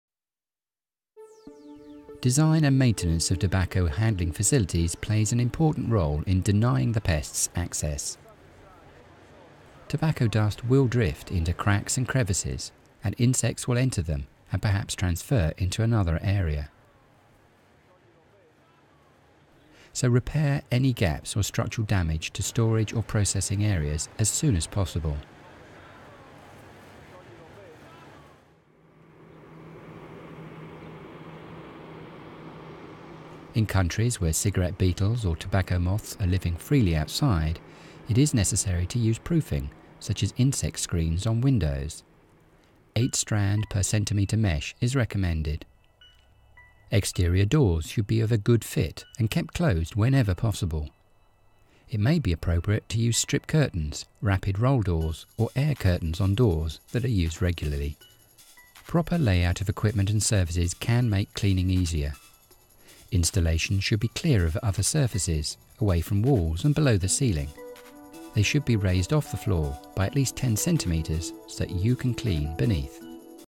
Tobacco Beetle Infestation Training Videos and DVD (English language Version)
A short sample of Part 5 of the DVD of the CORESTA Tobacco Beetle Infestation Videos.
These infestation training programmes were made for the CORESTA Infestation Sub-Group Committee working alongside leading tobacco manufacturers.  The videos are for training of Farmers, Leaf Processors, Tobacco Handlers and Tobacco Manufacturers. They show how to combat the cigarette beetle by good sanitation techniques, good storage methods, production building design, fumigation, freezing and a number of other methods.  The full training DVD (81 minutes) is available

Also available in French. English, Mandarin, Spanish, Russian